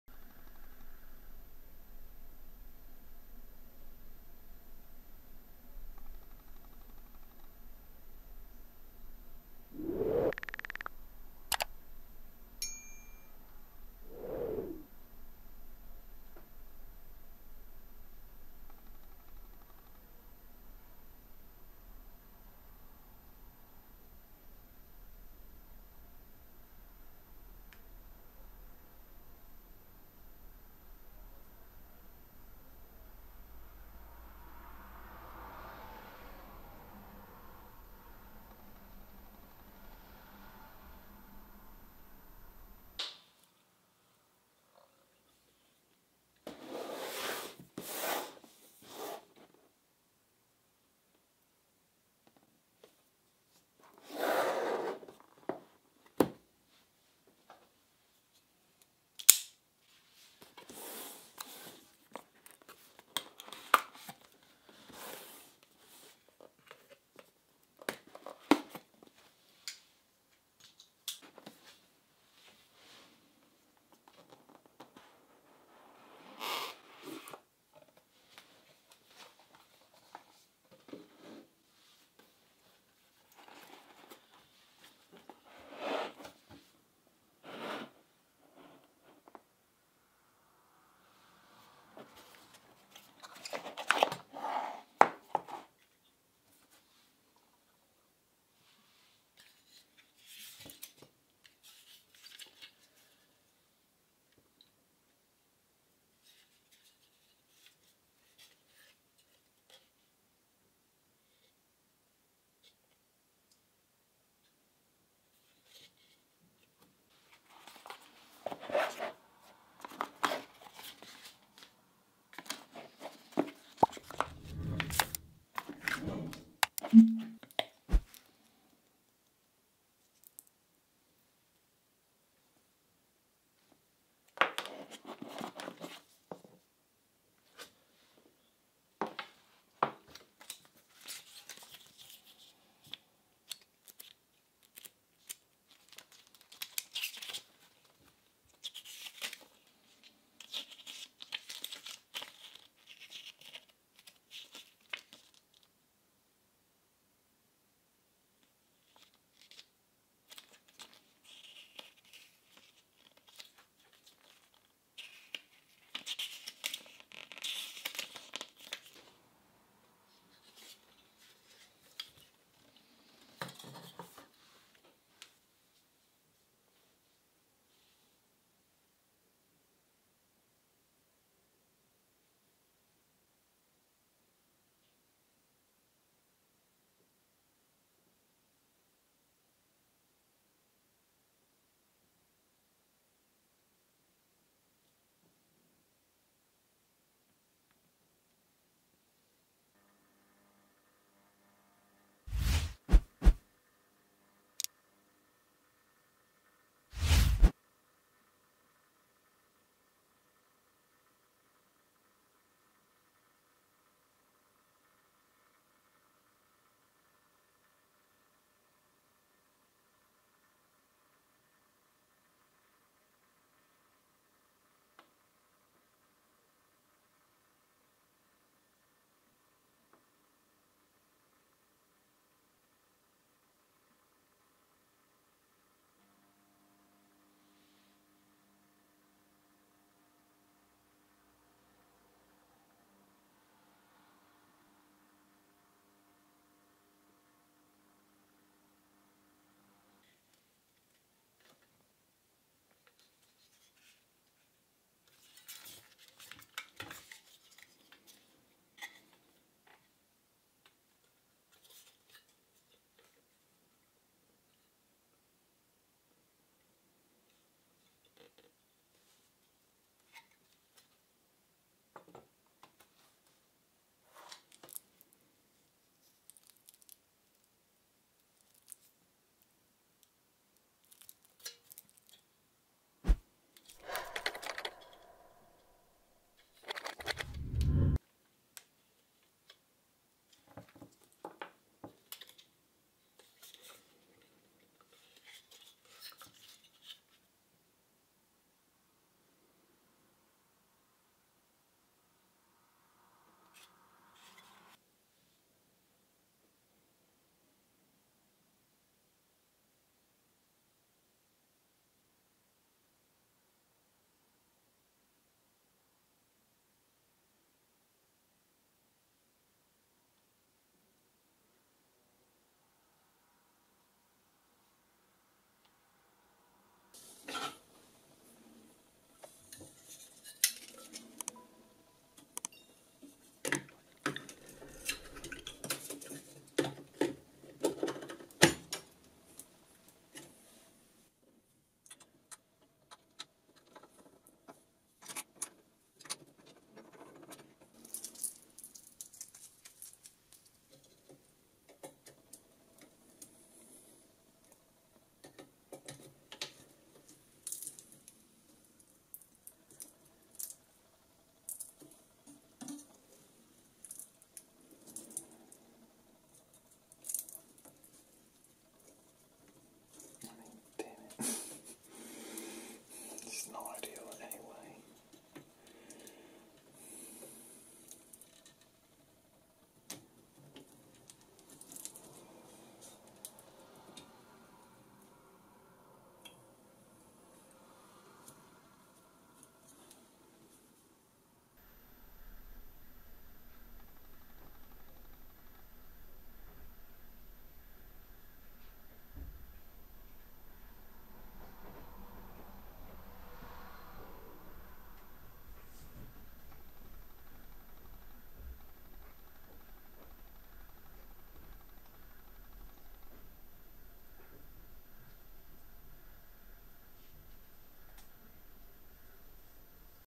ASMR unboxing Nvidia RTX 3060 TI Founder's Edition
Nvidia Geforce RTX 3060 TI specs:
GPU Engine Specs: NVIDIA CUDA® Cores 4864
Boost Clock (GHz) 1.67
Base Clock (GHz) 1.41
Memory Specs:
Standard Memory Config 8 GB GDDR6
Memory Interface Width 256-bit
Technology Support:
Ray Tracing Cores 2nd Generation
Tensor Cores 3rd Generation
NVIDIA Architecture Ampere
Microsoft DirectX® 12 Ultimate Yes
NVIDIA DLSS Yes
NVIDIA Reflex Yes
NVIDIA Broadcast Yes
PCI Express Gen 4 Yes
Resizable BAR Yes

If you'd like to see the full and proper videos, please see my other YouTube channel:

Recorded with:
Panasonic Lumix GH4
Panasonic Lumix G Vario 12-32 mm Lens
Panasonic Lumix G Macro Lens
Blue Yeti mic

PLEASE NOTE